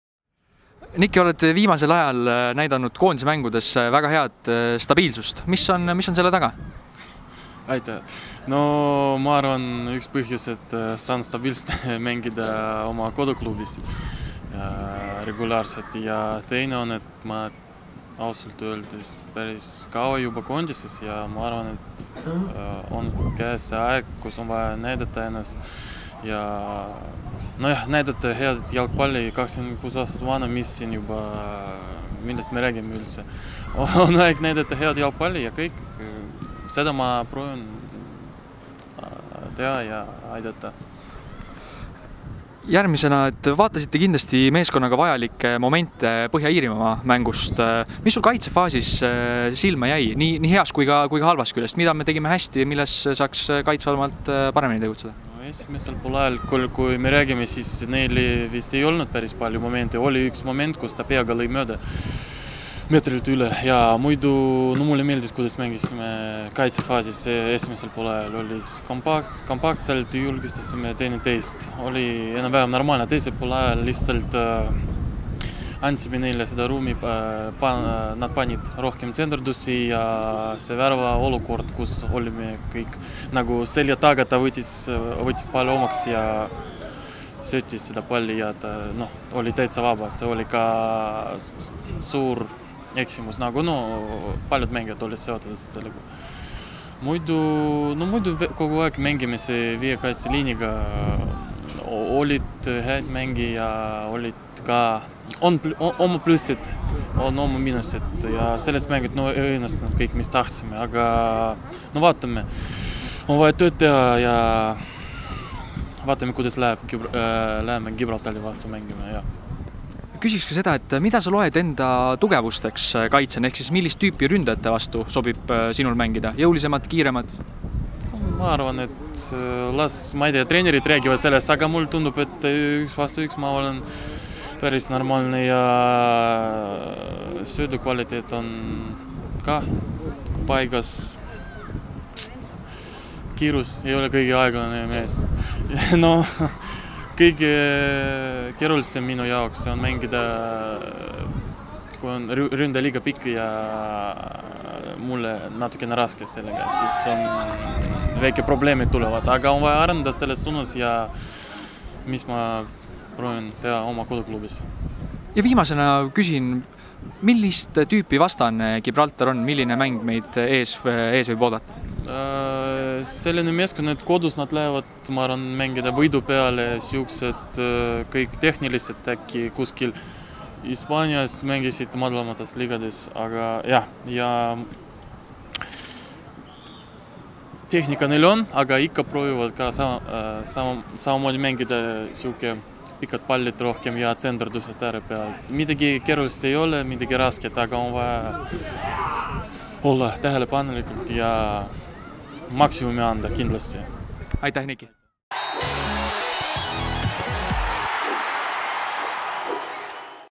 Nikita Baranoviga kaitsemängust ja Gibraltarist (25.03.2019)
Meeste koondise kaitsja Nikita Baranov vaatab tagasi EM-valiksarja avamängule Põhja-Iirimaal ja iseloomustab järgmist vastast Gibraltarit.